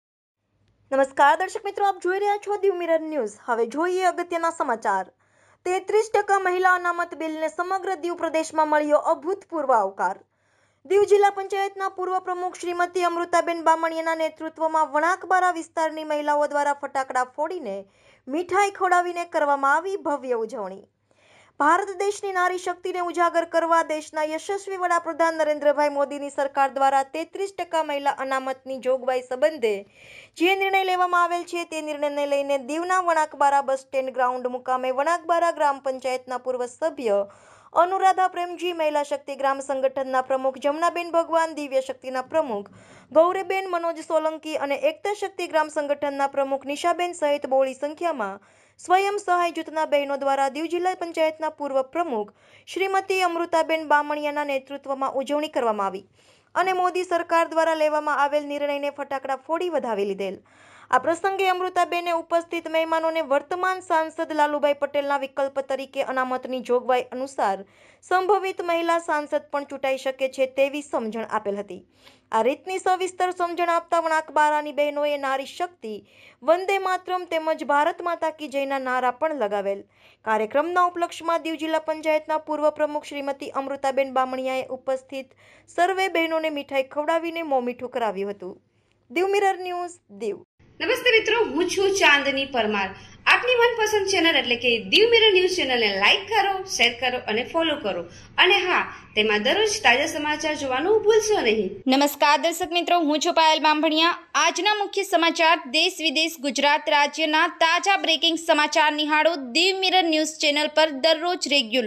૩૩% મહિલા અનામત બિલને સમગ્ર દિવ પ્રદેશમાં મળ્યો અભૂતપૂર્વ આવકાર | Diu | Una
૩૩% મહિલા અનામત બિલને સમગ્ર દિવ પ્રદેશમાં મળ્યો અભૂતપૂર્વ આવકાર | Diu | Una